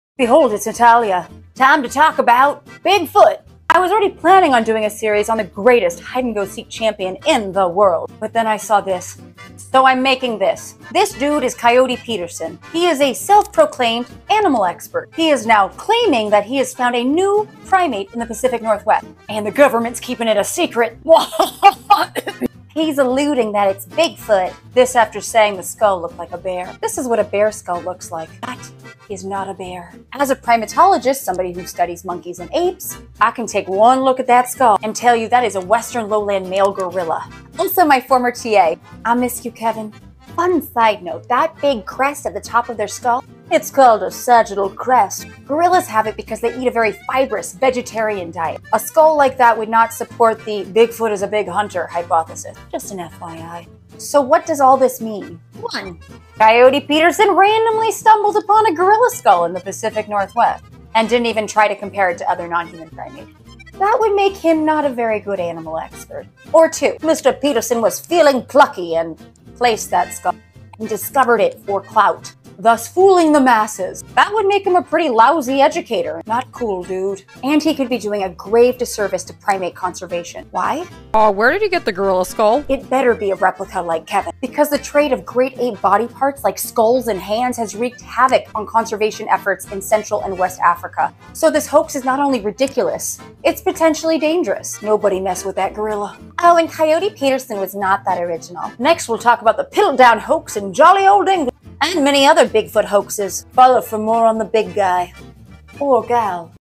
Debunking Coyote Peterson’s Bigfoot Hoax
Let’s talk about the dangers of Coyote Peterson’s Bigfoot “discovery” (read: ridiculous hoax).